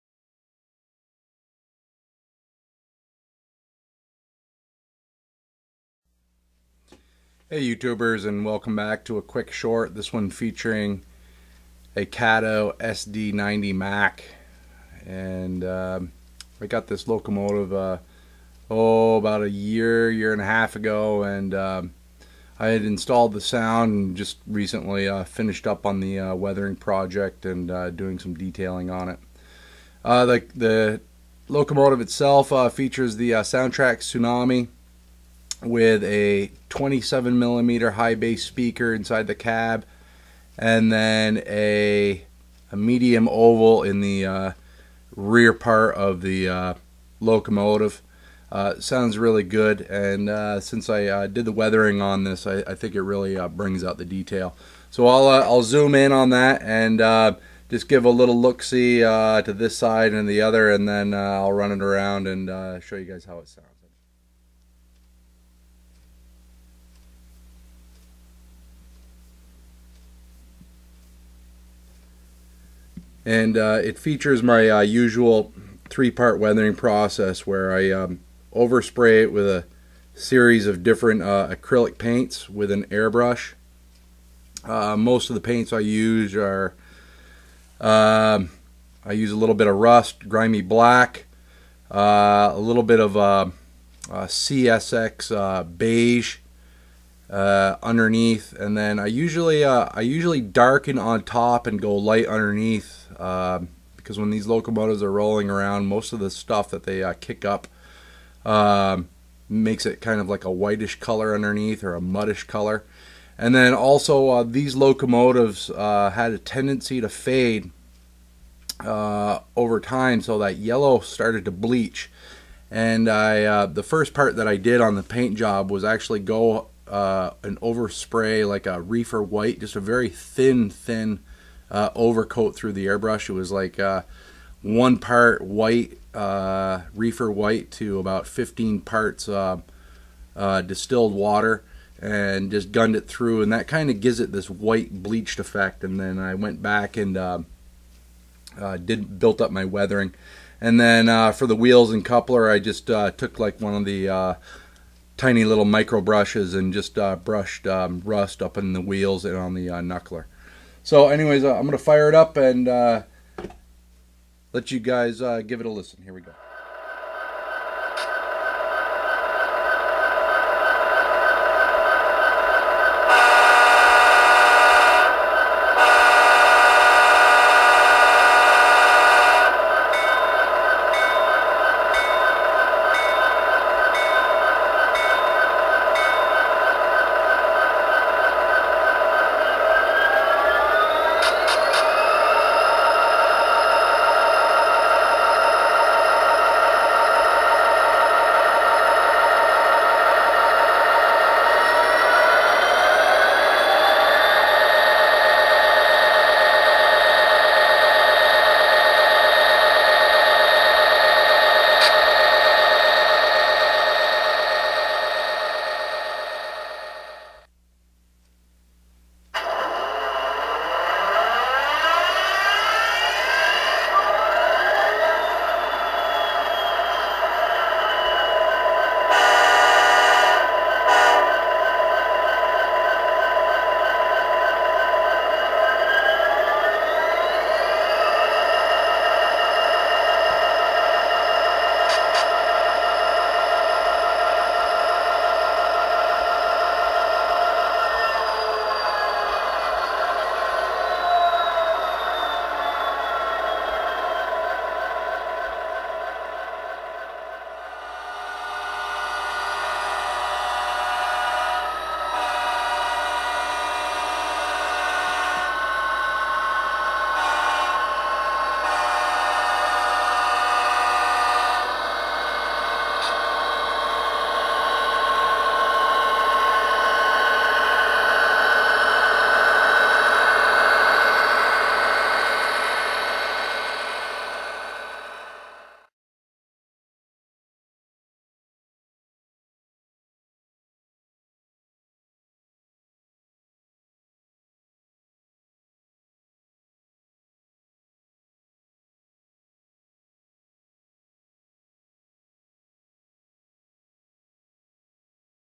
Kato SD90MAC with SoundTraxx Tsunami
Kato with Tsunami installed, weathered. It has two speakers wired in parallel; 27mm HB speaker in the cab and a 34.7x20mm oval speaker in the rear. The light pipes were removed and 0603 LEDs were used for RL/HL and 0402 LEDs for the ditches.